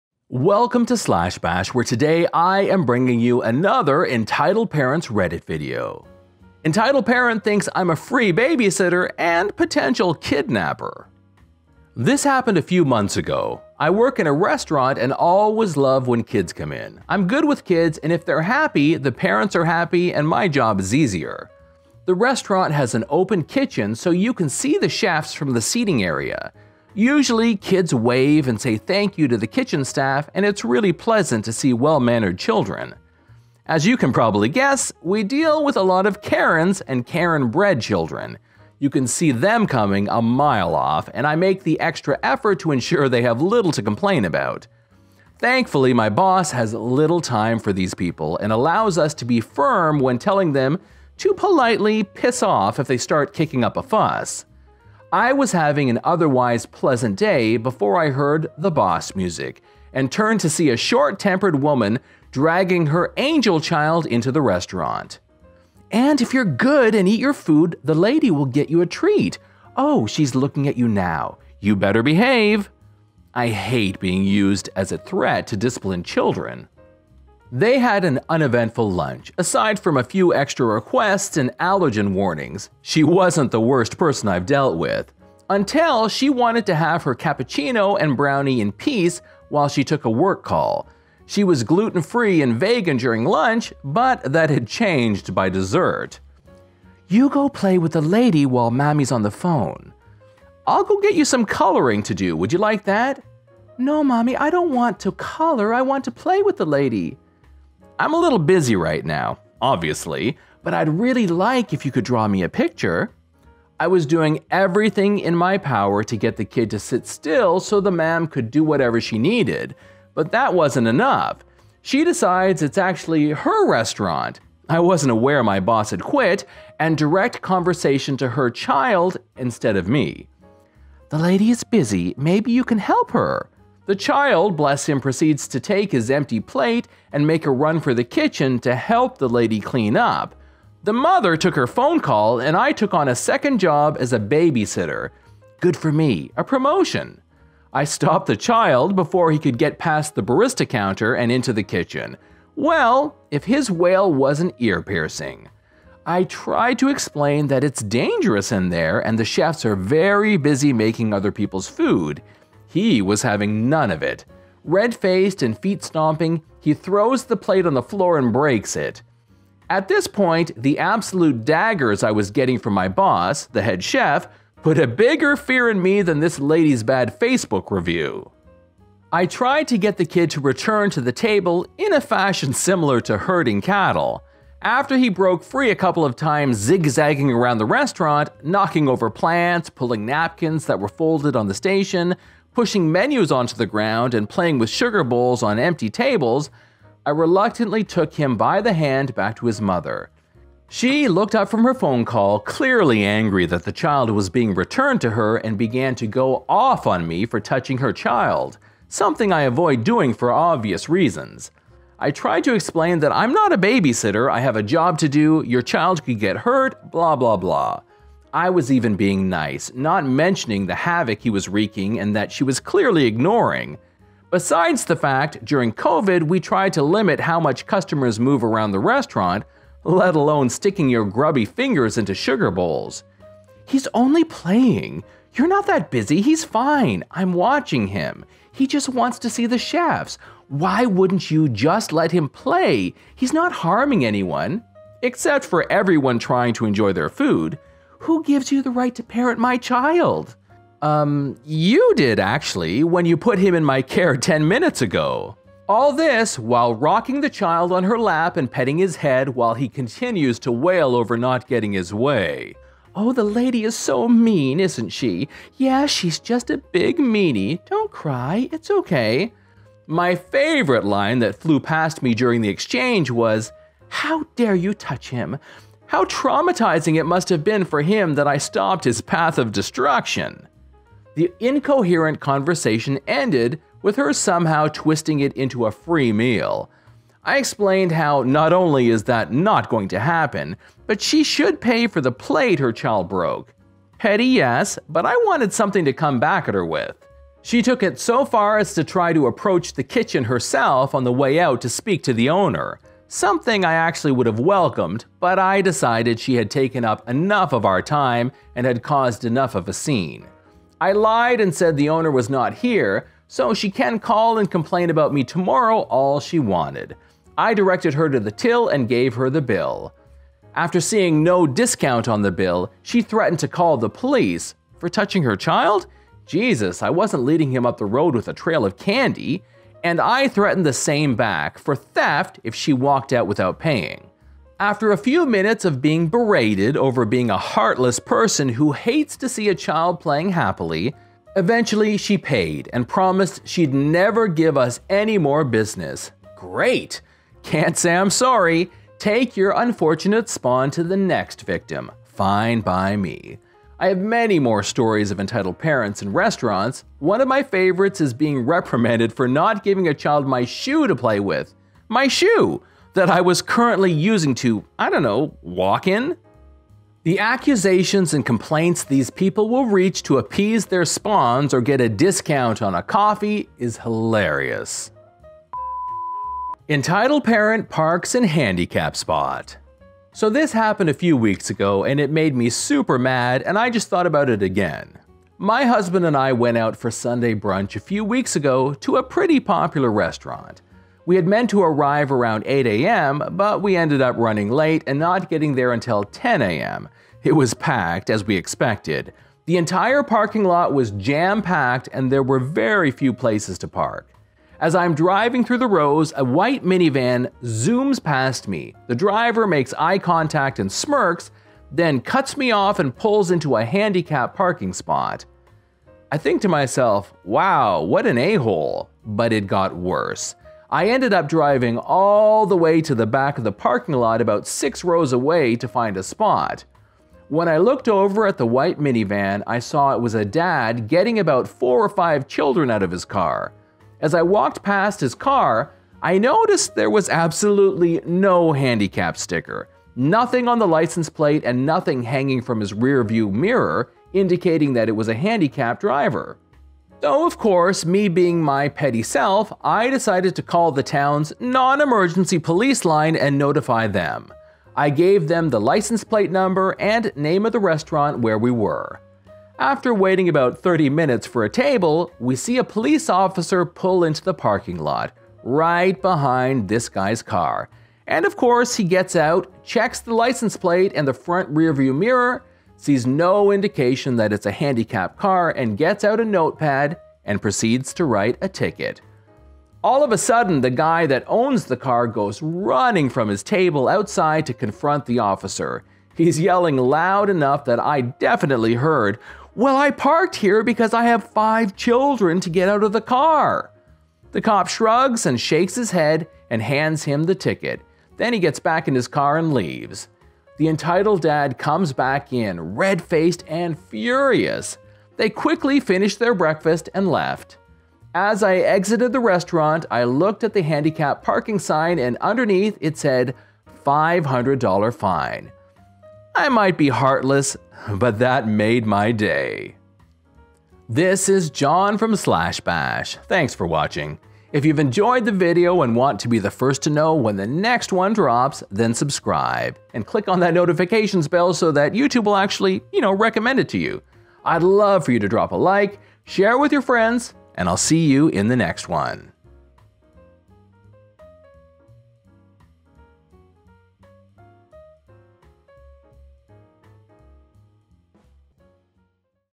r/entitledparents // EP thinks I am a free babysitter, and potential kidnapper 👪 REDDIT
Entitled mother confuses restaurant with day care center

🎙️ Microphone: Snowball Blue
💣Pop Filter: GOMILE
🚏 Mic Stand with Boom
💻 Lenovo Yoga 730